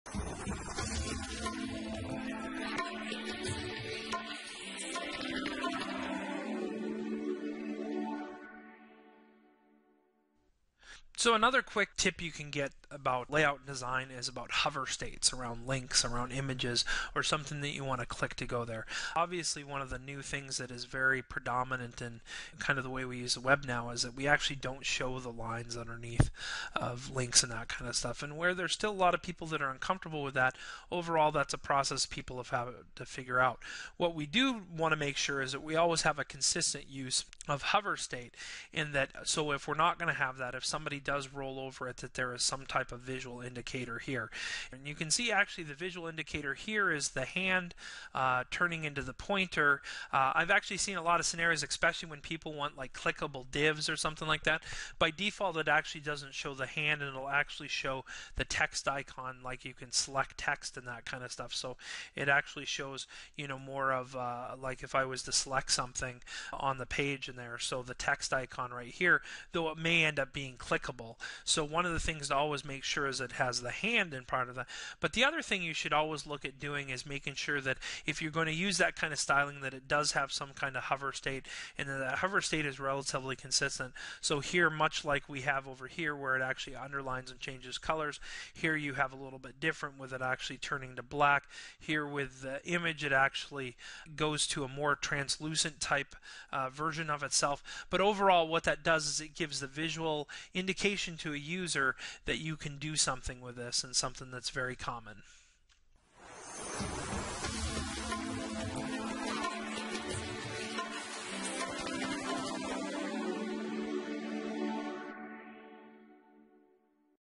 101 SharePoint Usability Tips - # 17 The Cursor's Hover State
Placing your cursor on a clickable item should change the hover state of your cursor so users can easily identify what is clickable on any given page. If the hover state does not change users may not realize the site's full functionality and could overlook important components of the site.